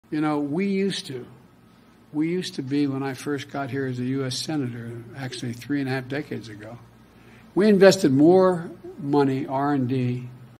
Joe Biden Says 1973 Was “Three And A Half Decades Ago”
President Joe Biden said 1973 was “three and a half decades ago,” during a speech to the staff at the Office of the Director of National Intelligence on 7/27/2021.